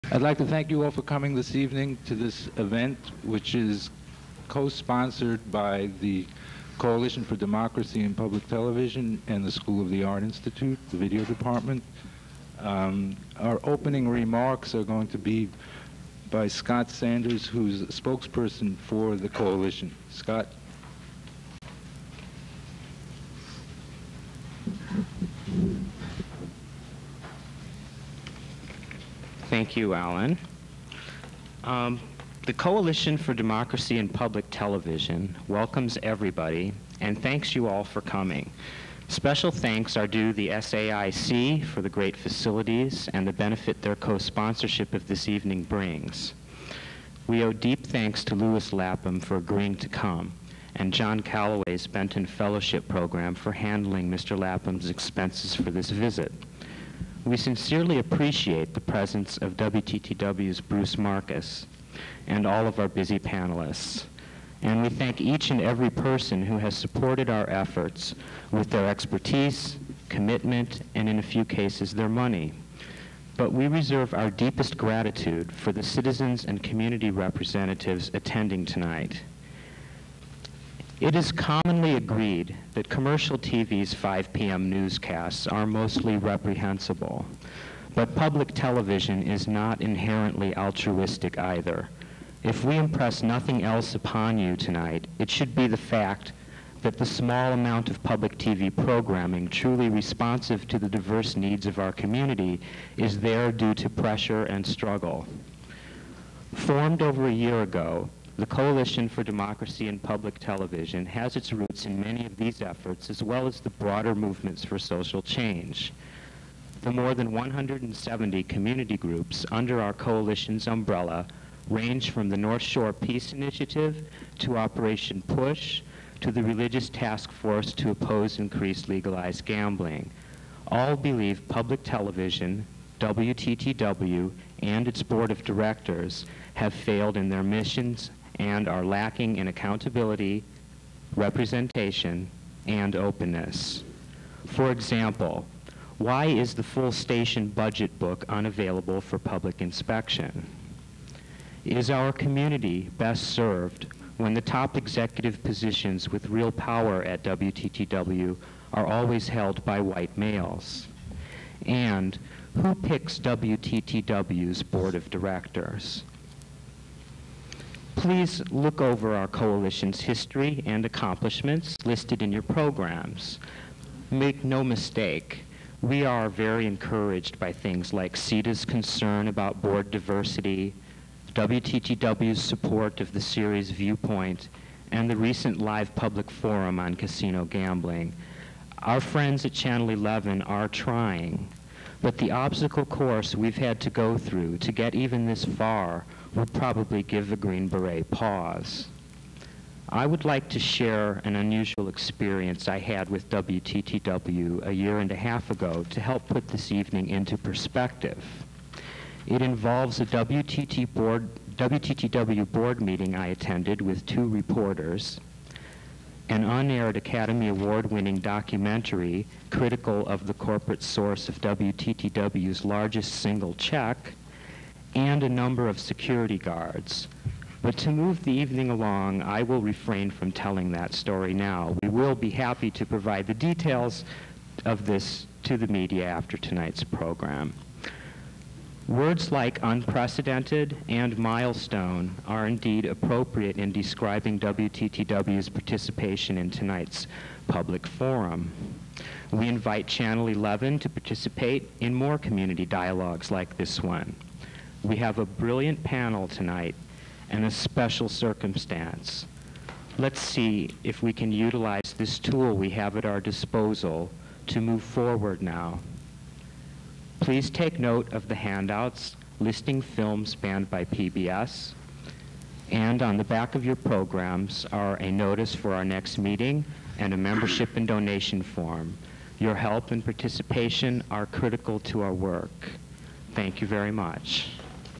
Scott Sanders speaks at the WTTW SAIC panel
. . . . .this event was organized by the Coalition for Democracy in Public Television at the School of the Art Institute of Chicago and was held on May 2, 1994 . . . . . host: Dalida Maria Benfield - Women in the Director's Chair . . . . .  keynote speaker: Lewis Lapham - editor of the John D. and Catherine T. MacArthur Foundation's +Harper's magazine . . . . . panelists: Bruce Marcus - director of public relations, public station WTTW-TV . . . . . Gordon Quinn - Kartemqun Films, exec. prod. "Hoop Dreams" . . . . .  Atty. Janette Wilson, executive director Operation PUSH . . . . . Carlos Tortolero - director, Mexican Fine Arts Museum . . . . . introduction by Allan Siegel, professor in the film department of SAIC . . . . . opening remarks by Scott Sanders, elected representative of the Coalition for Democracy in Public TV